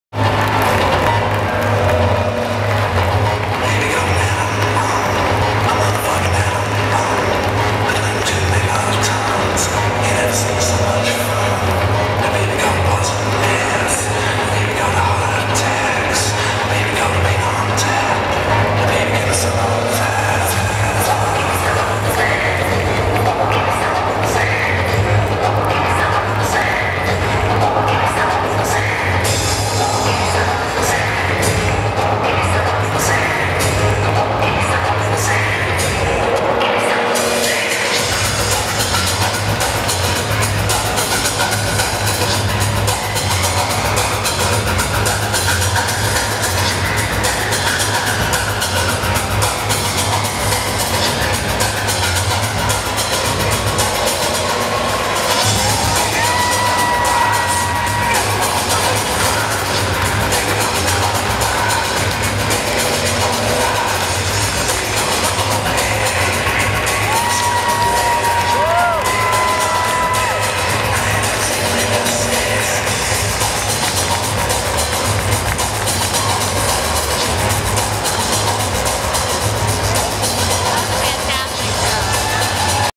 MILA by Mila Hermanovski | Fall/Winter 2012 | LAFW
Project Runway All-Star & Season 7 finalist, Mila Hermanovski, presented her Fall/Winter 2012 MILA collection as part of Style Fashion Week on Wednesday, March 13th at the Vibiana in Los Angeles. Here's a glimpse of the collection by way to the finale.